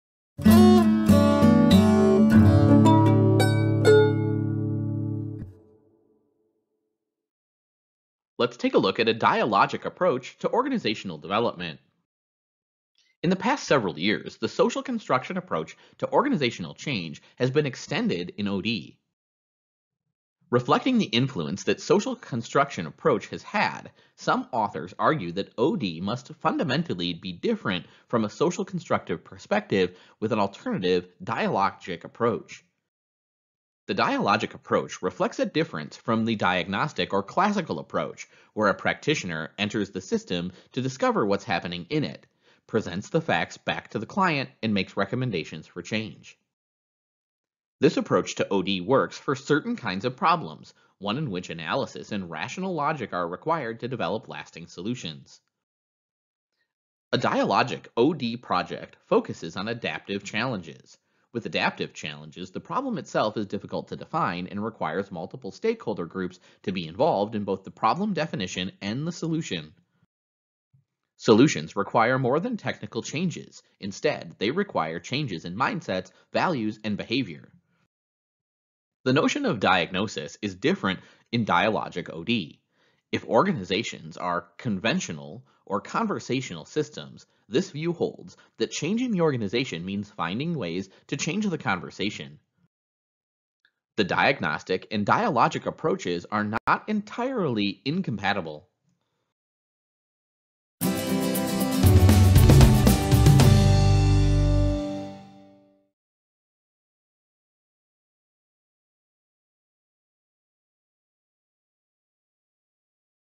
Dialogic Approach to Organizational Development
In the past several years, the social construction approach to organizational change has been extended in OD. Reflecting on the influence that the social construction approach has had, some authors argue that OD must be fundamentally different from a social construction perspective with an alternative dialogic approach.  This dialogic approach reflects a difference from the diagnostic or classic approach.

A practitioner enters the system to discover what is happening in it, presents the facts back to the client, and makes recommendations for change. This approach to OD works for certain kinds of problems, ones in which analysis and rational logic are required to develop lasting solutions. A dialogic OD project focuses on adaptive challenges. With adaptive challenges, the problem itself is difficult to define and requires multiple stakeholder groups to be involved in both the problem definition and the solution.

Solutions require more than technical changes; instead, they require changes in mindsets, values, and behaviors. Dialogic OD is based on a different set of foundational assumptions and practices in order to address these adaptive challenges. The notion of diagnosis is different in dialogic OD. If organizations are conversational systems, this view holds, then changing the organization means finding ways to change the conversation. The diagnostic and dialogic approaches are not entirely incompatible.